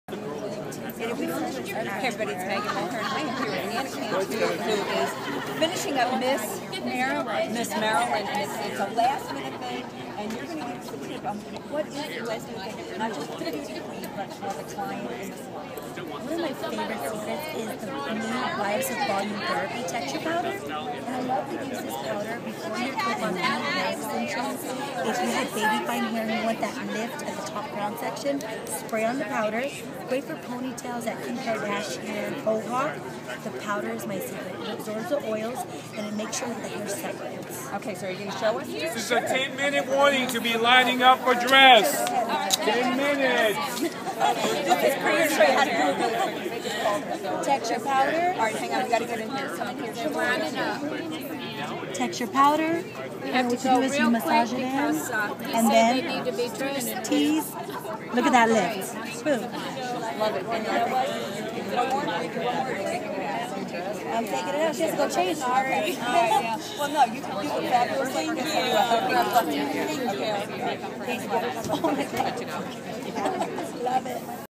Anna Cantu at Miss USA
Anna Cantu demos Volumizing Therapy powder at Miss USA